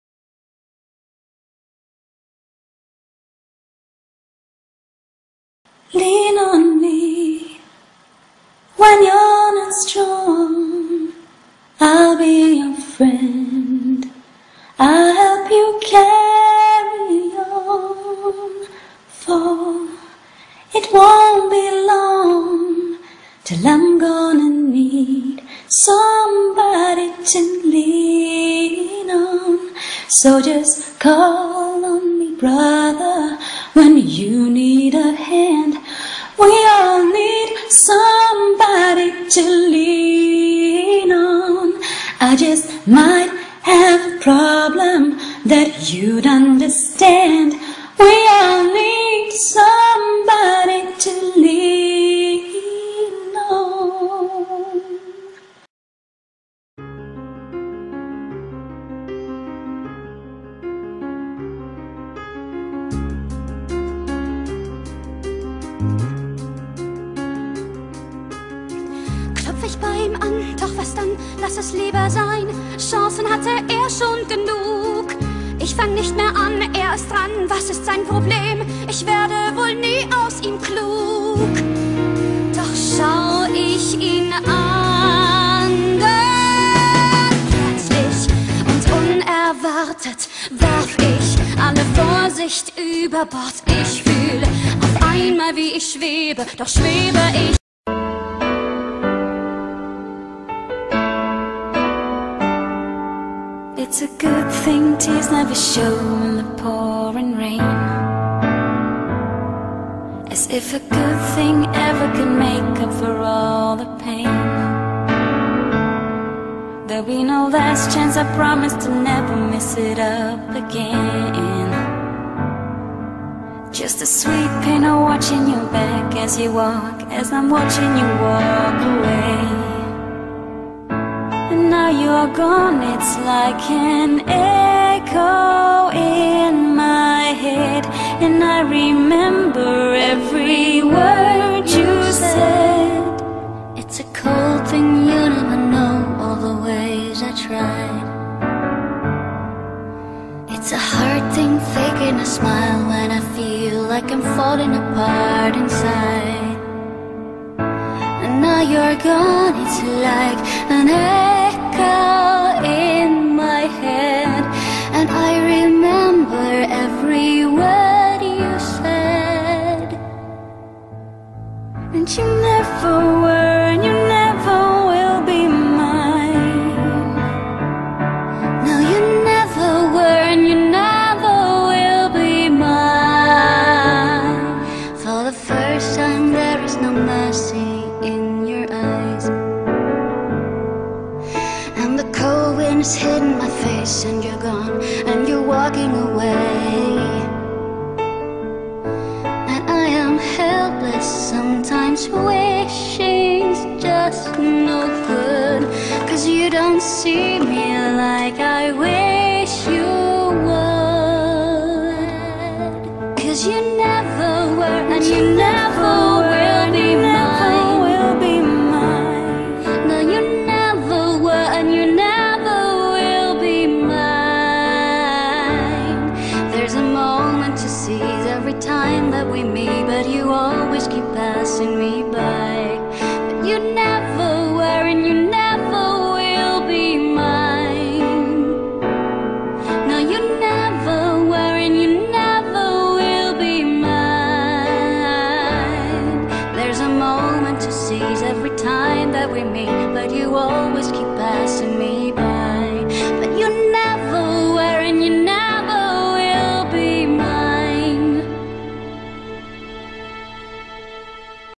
Lucy Scherer and Kasia Borek - Jemma
a fanvideo about Lucy and Kasia and with the Jemma story